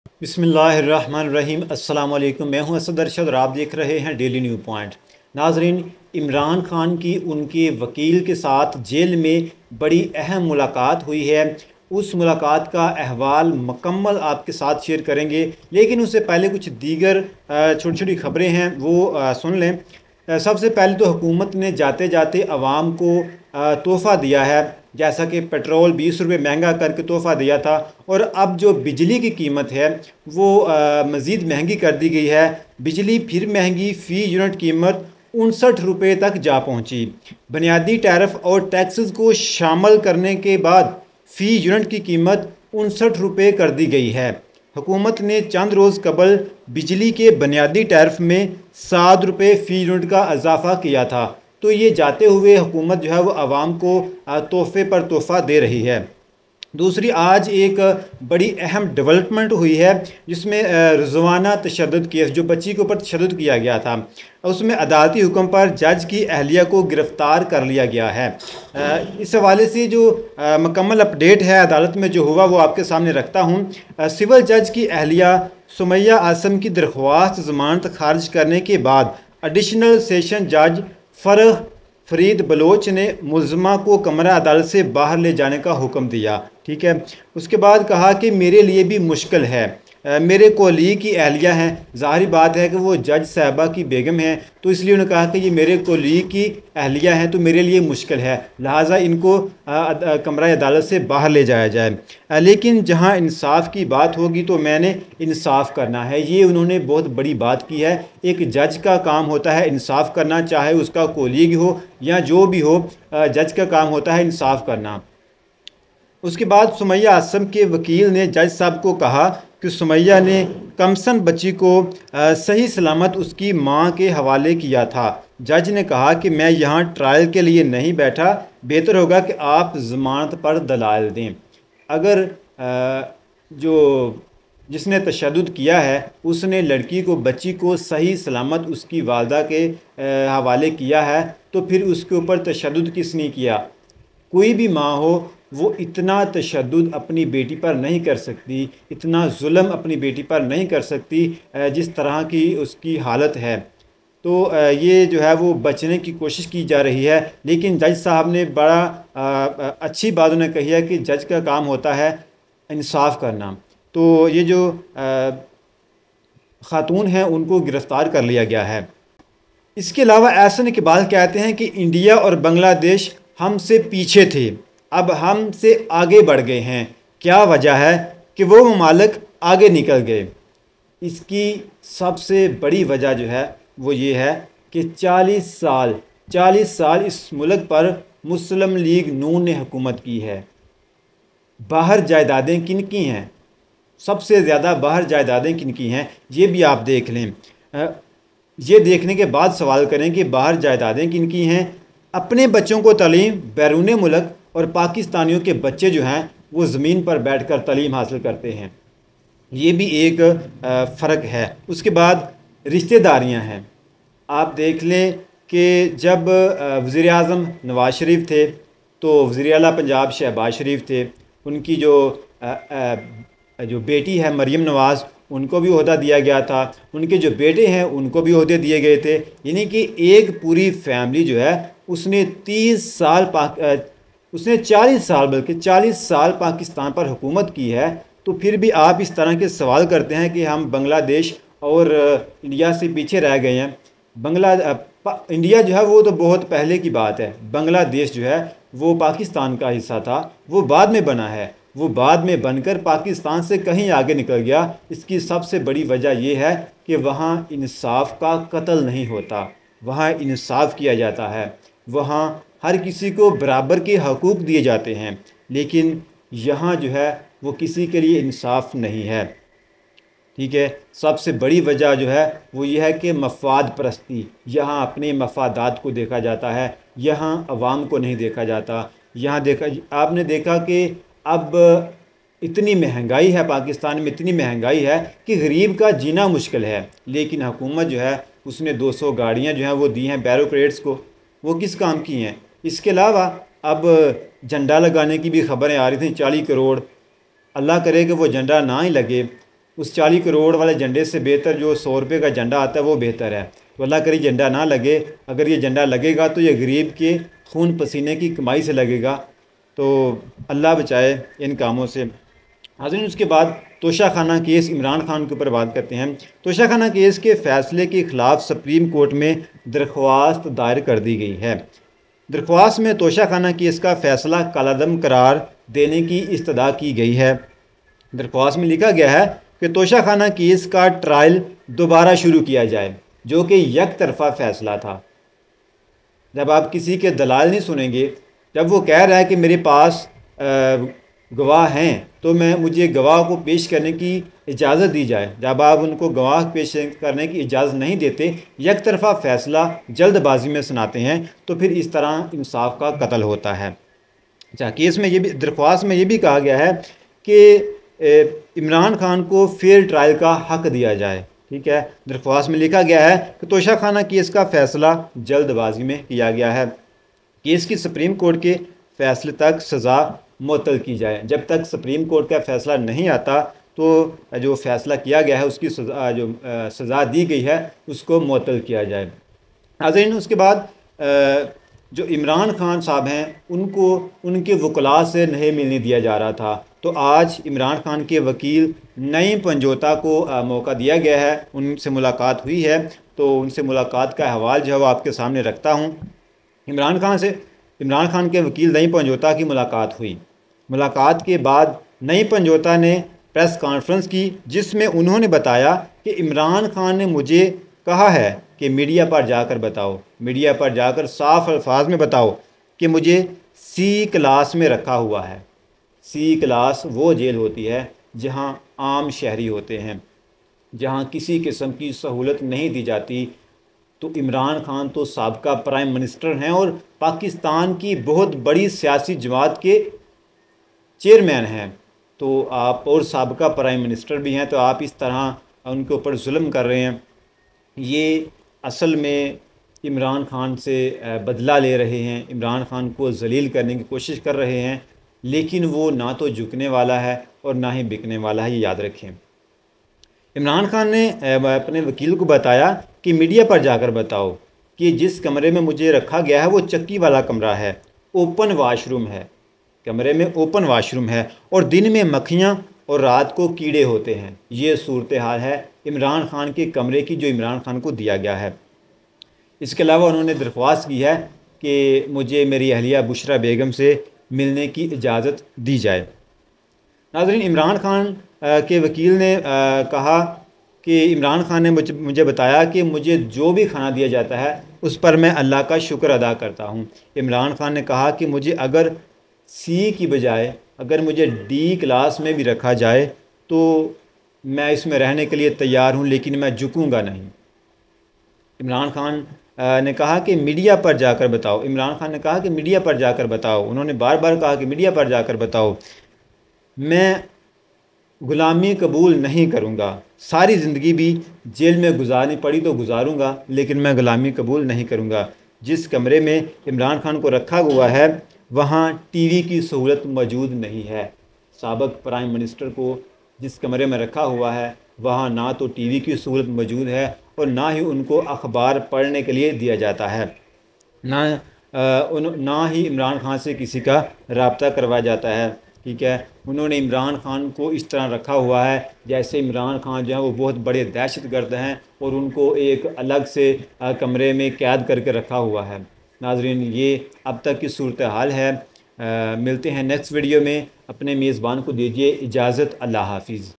عمران خان اور وکیل کی جیل میں ملاقات کے دوران کیا باتیں ہوئیں ؟ سب کھل کر سامنے آگیا! Imran Khan
عمران خان اور وکیل کی جیل میں ملاقات کے دوران کیا باتیں ہوئیں ؟ سب کھل کر سامنے آگیا! Imran Khan Latest Update